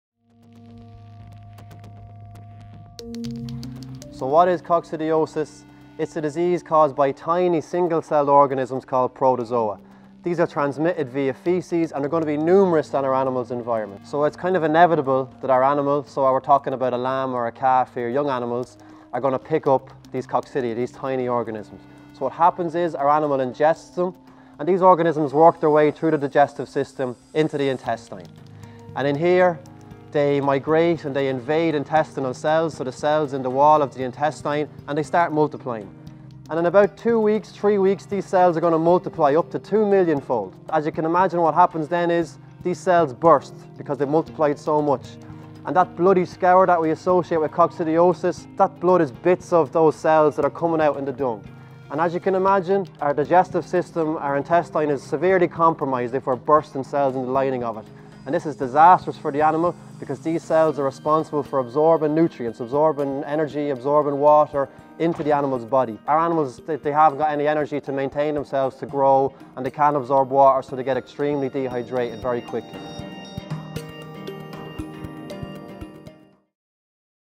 Cocci Series (IE & UK) - What is coccidiosis?
Technical Specialist Ciarán Lenehan gives an overview of coccidiosis in livestock.
(IE & UK)